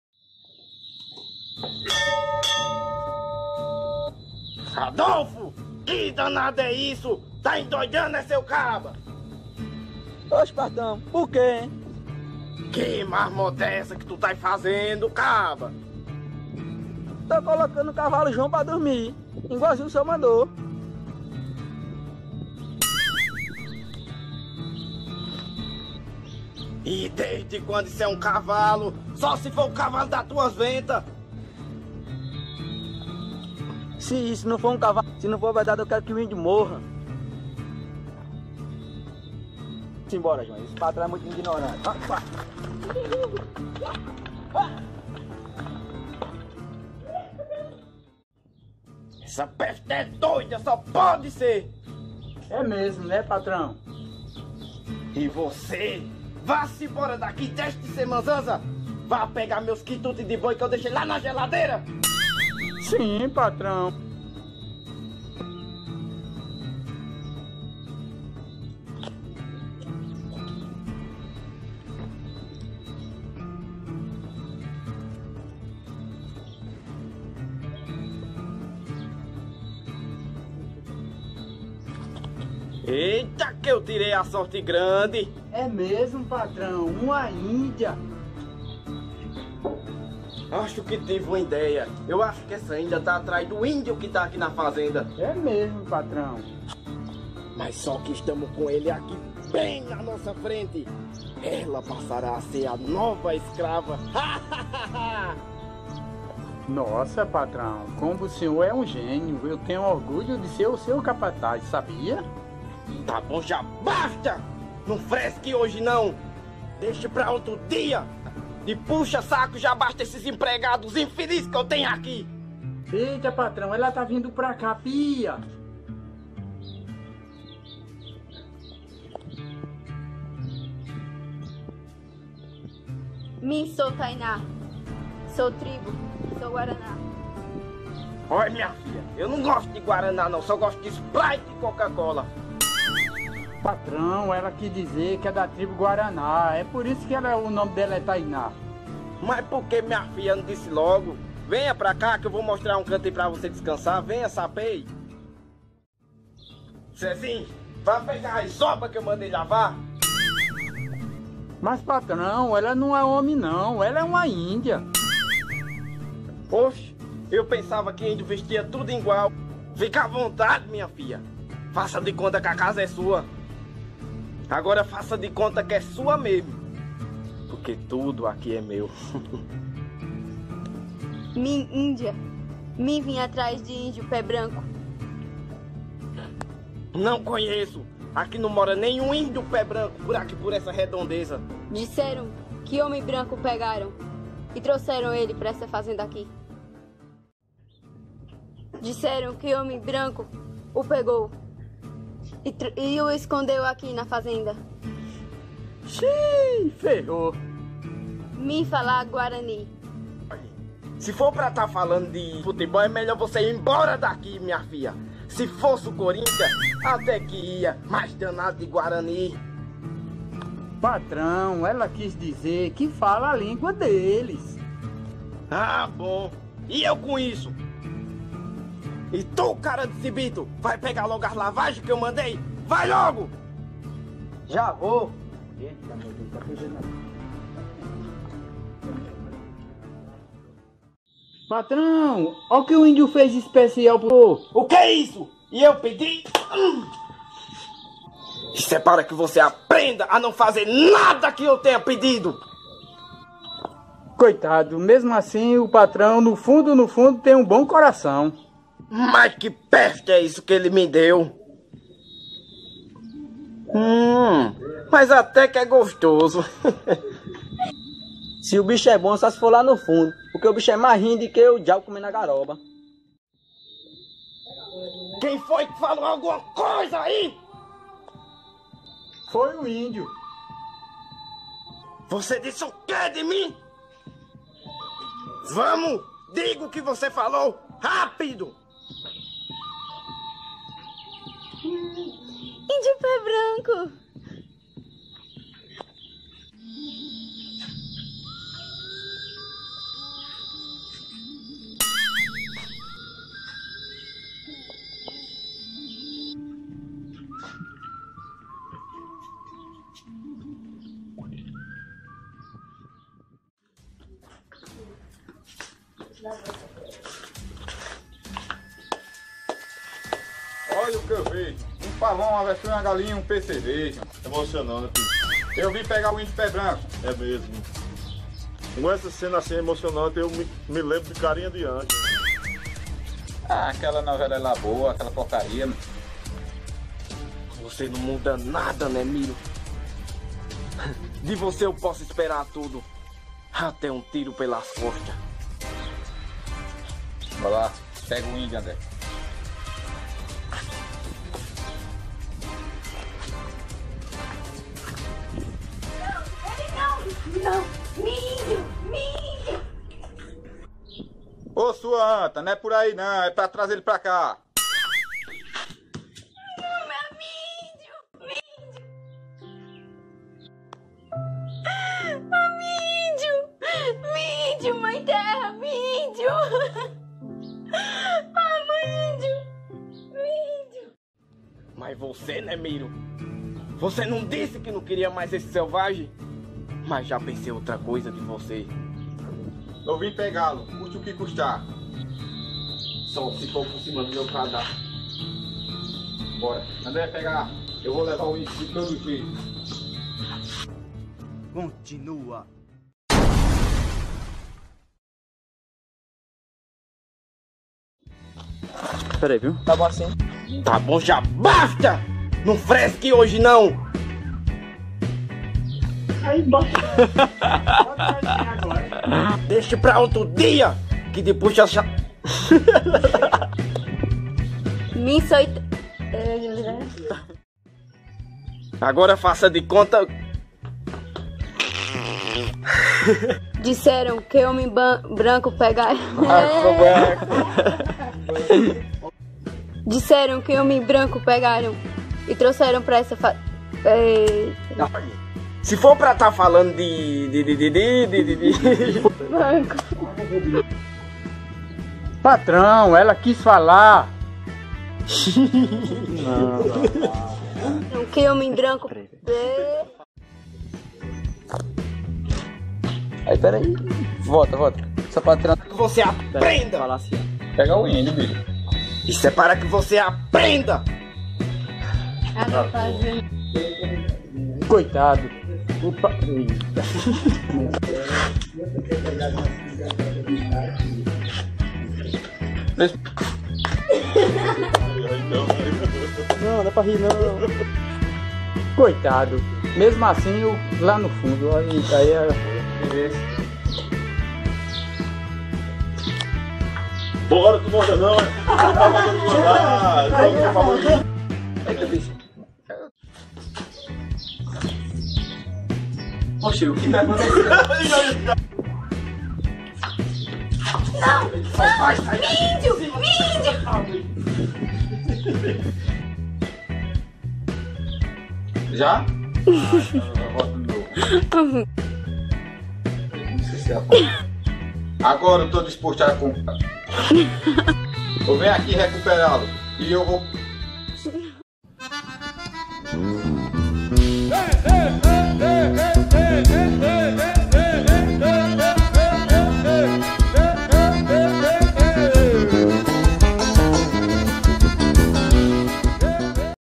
A FAZENDA - DE VOLTA AO VELHO OESTE
Na pacata fazenda do Coronel Rodolfo. Seu capataz Adolfo resolve cavalgar com o cavalo Jão. Zezin Zanôi como sempre bajula o seu patrão Rodolfo, enquanto o índio Pé Branco se encontra refém de sua própria ignorância e inocência natural. Do nada, aparece Tainá, uma linda índia da tribo Guaraná que aprendeu o português para resgatar seu companheiro de tribo: O índio Pé Branco. Após o reencontro emocionante dos dois, eis que aparece Miro e seu amigo André Bililong atrás do índio. Não deixe de dá o seu like e se você ainda não é inscrito, dá uma força aí e se inscreve. Beleza? Valeu galera!!